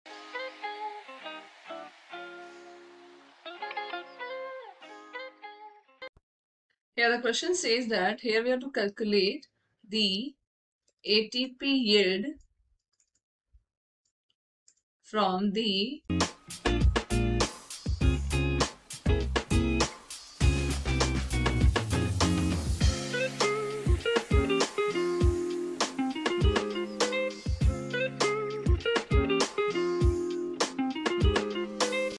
calculate the ATP yield from the oxidation of histidine , taking into account the energy needed to …
calculate the ATP yield from the oxidation of histidine , taking into account the energy needed to activate the fatty acid and transport it into the mitochondria. please write energy producing steps written out.